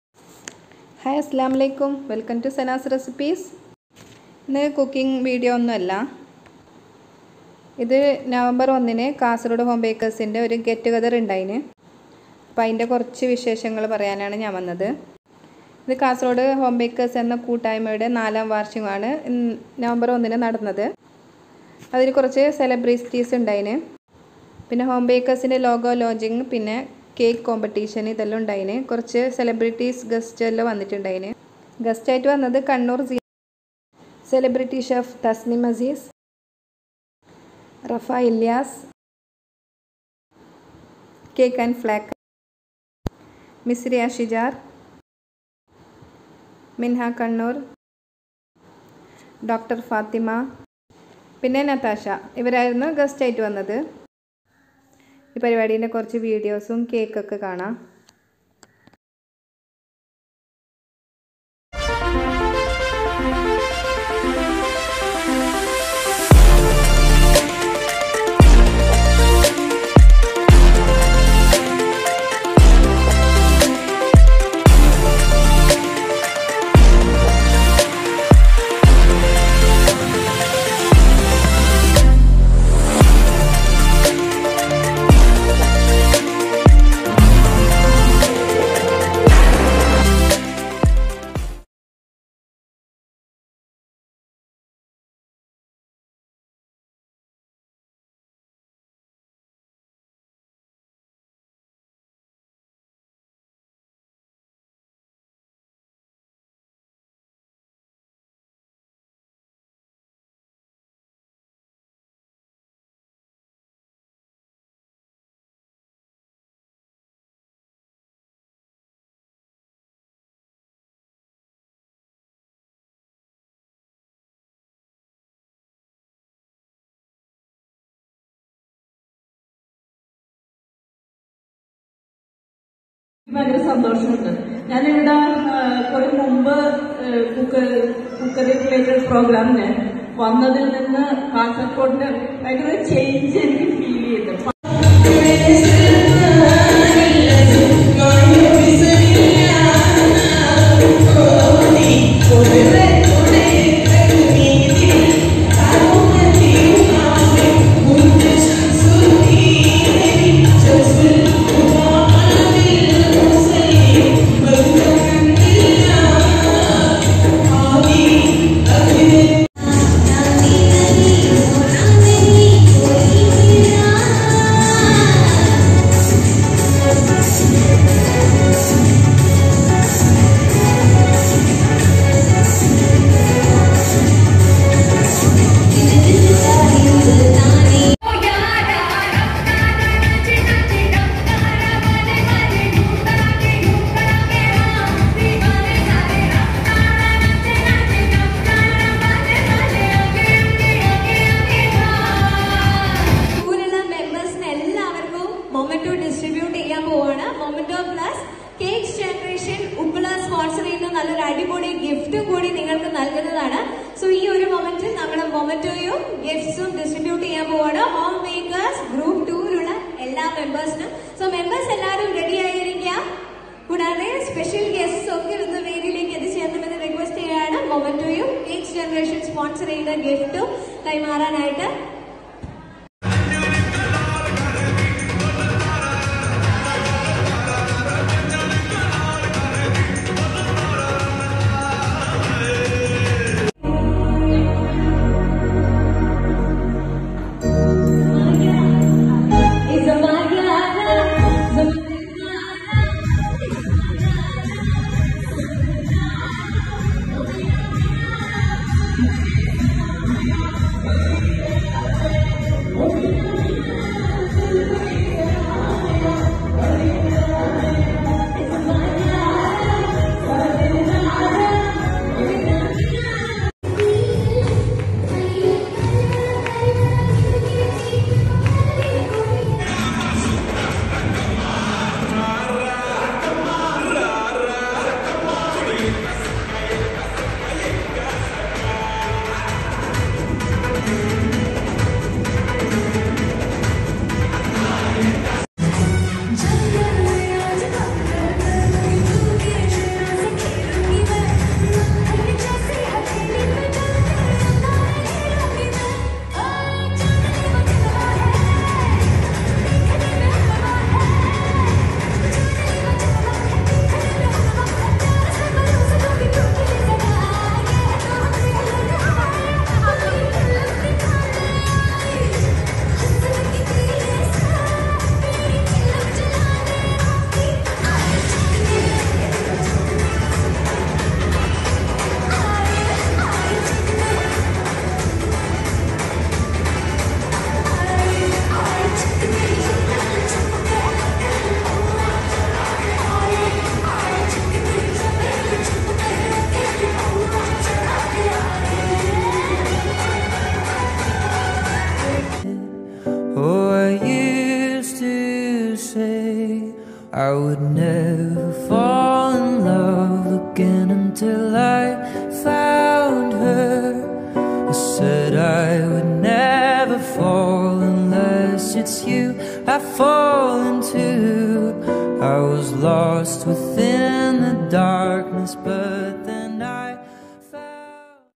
Kasaragod Homebakers ൻ്റേ Taste Of Success ഒരു അടിപൊളി Getogether Cake competition| Getogether vlog
Kasaragod Homebakers Taste Of Success ഒരു അടിപൊളി Getogether Cake competition| Getogether vlog